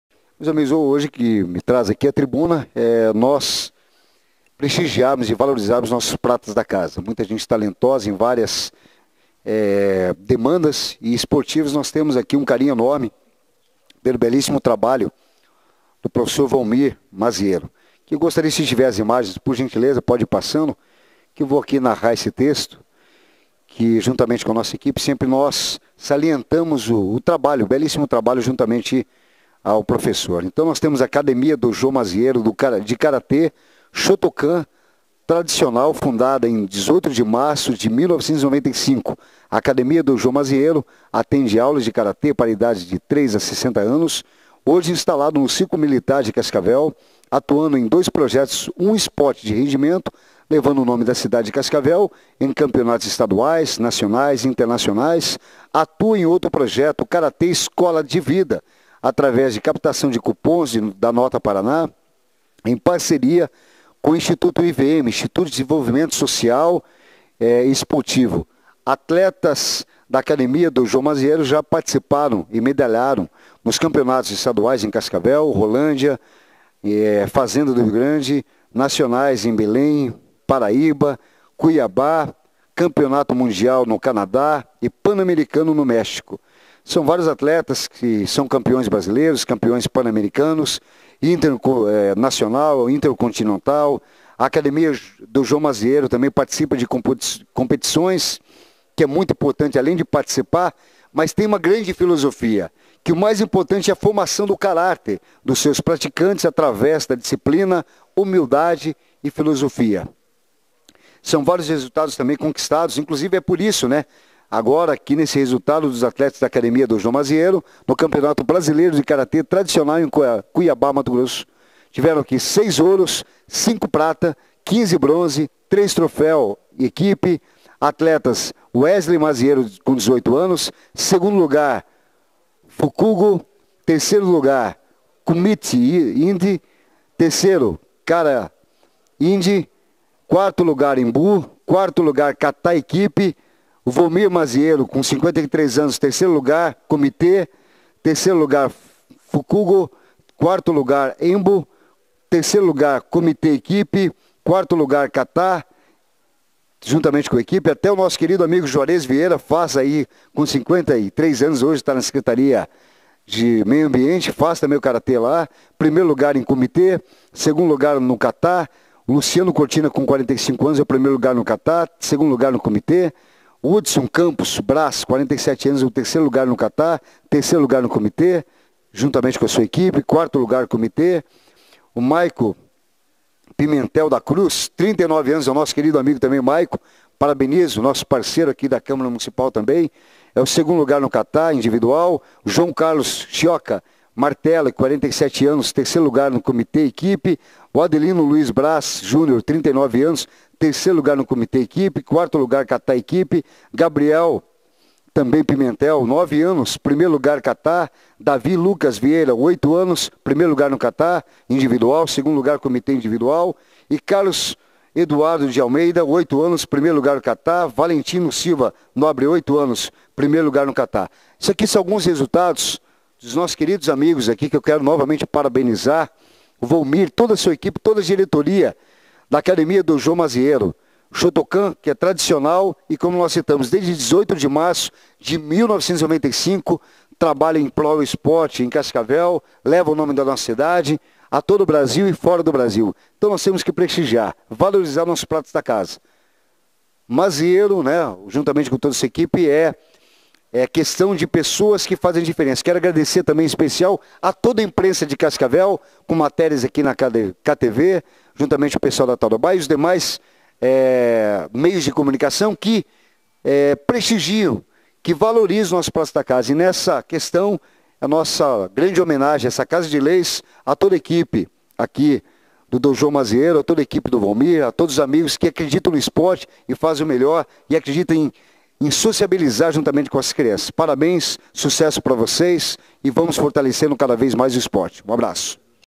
Academia Dojo Maziero conquistas várias medalhas em Cuibá - MT
Quero parabenizar a academia Dojo Maziero - Karatê Shotokan Tradicional na pessoa do Sensei Volmir Maziero treinador de toda a equipe cascavelense que viajou para Cuiabá - MT no mês de julho, onde disputaram o Campeonato Brasileiro de Karatê Tradicional.

De lá trouxeram para Cascavel: 06 Ouro / 05 Prata / 15 Bronze / 03 Troféus equipe. Acompanhe a resultado de cada atleta:

Wesley Maziero 18 anos
2° lugar Fukugo
3° lugar Kumite ind.
3° kara ind.
4° lugar Enbu
4° lugar kata equipe

Volmir Maziero 53 anos
3° lugar kumite ind.
3° lugar  Fukugo
4° lugar  Enbu
3° lugar kumite equipe
4° lugar kata equipe

Juarez Vieira 53 anos
1° lugar kumite ind
2° lugar kata ind

Luciano Cortina 45 anos
1° lugar kata ind
2° lugar kumite ind

Hudson Campos Braz 47 anos
3° lugar kata ind
3° lugar kumite equipe
4° lugar kumite ind

Maiycon Pimentel da Cruz 39 anos
2° lugar kata ind

Joao Carlos Chiocca Martelli 47 anos
3° lugar kumite equipe

Adelino Luiz Braz Junior 39 anos
3° lugar kumite equipe
4° lugar kata equipe

Gabriel Pietracatella Pimentel 09 anos
1° lugar kata ind

Davi Lucas Vieira 08 anos
1° lugar kata ind
2° lugar kumite ind

Carlos Eduardo Hoffmaister de Almeida  08 anos
1° lugar kata ind

Valentino Silva Nobre 08 anos
1° lugar kata ind